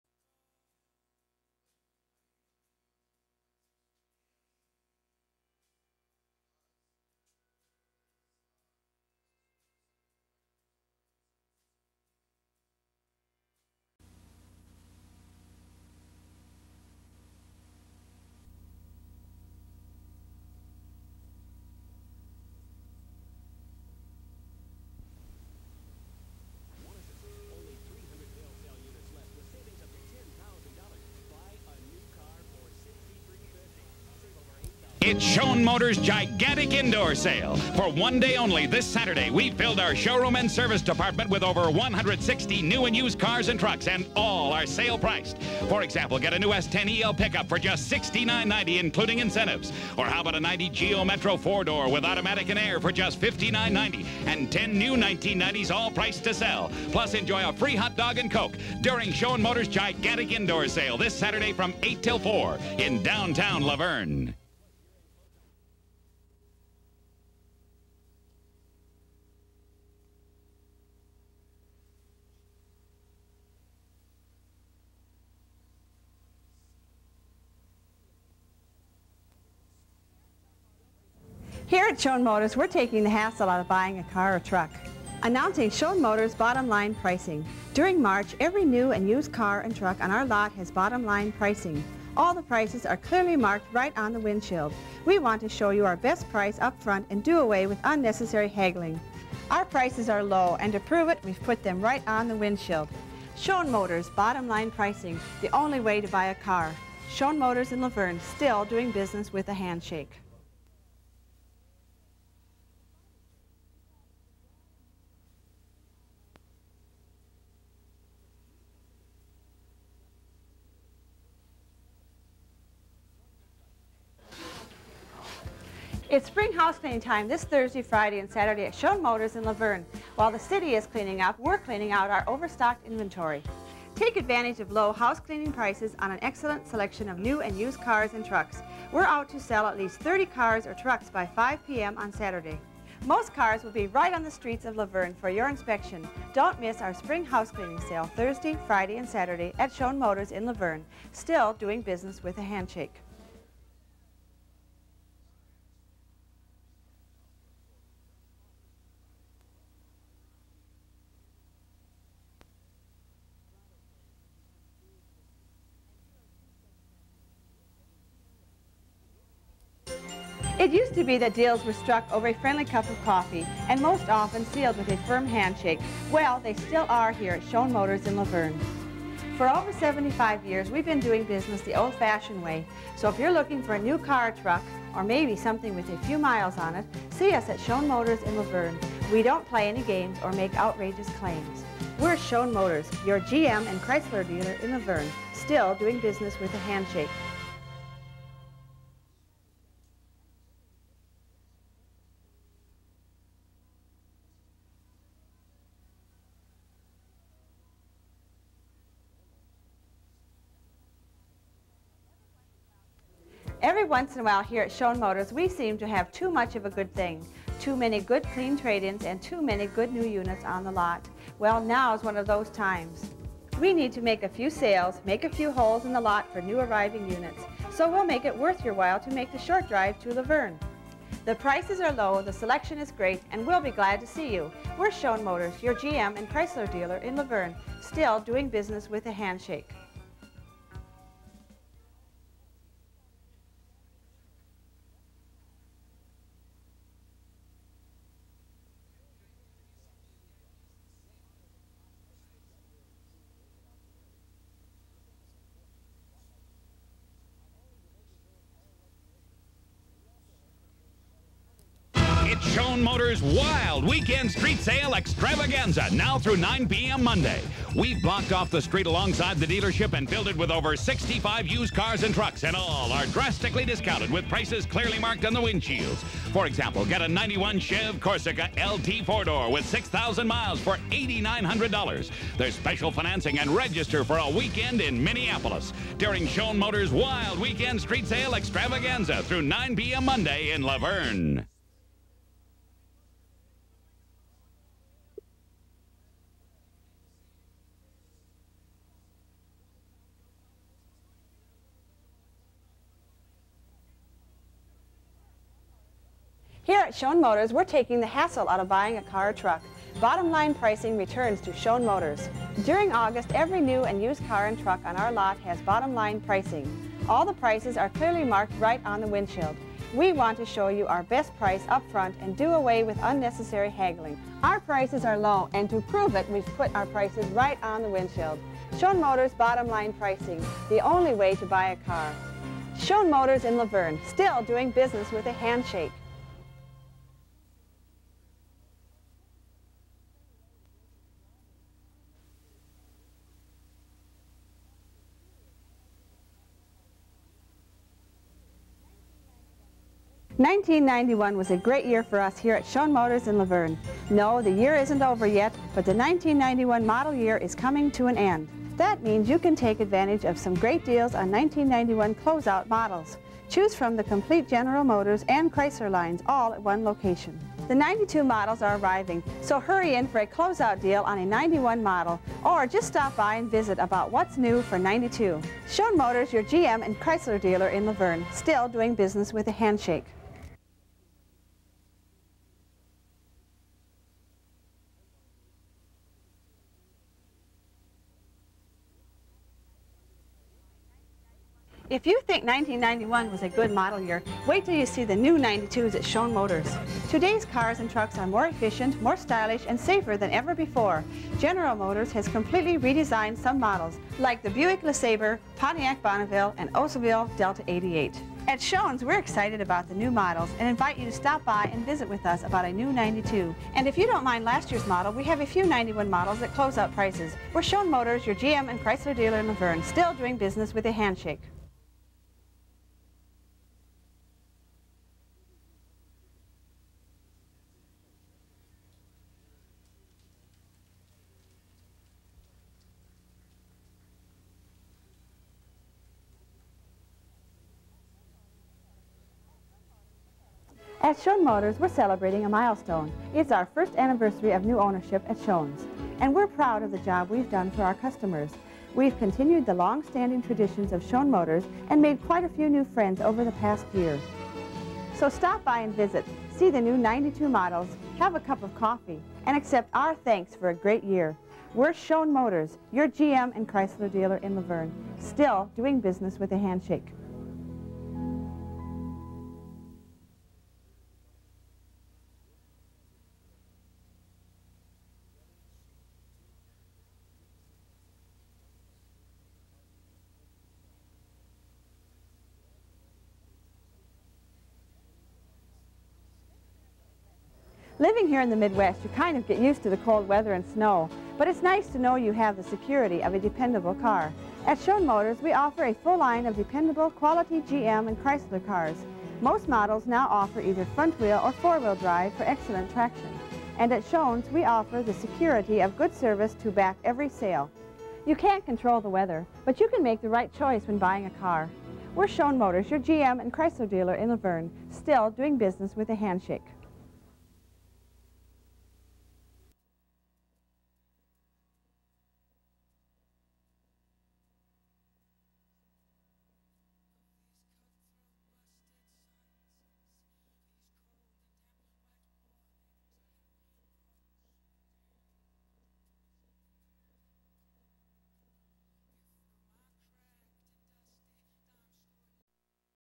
1991 Papik Motors TV Spots
Turn back time and enjoy these TV spots from 1991.  These are for Schoon Motors and in the old location in downtown Luverne.  Janine Papik is featured in many commercials.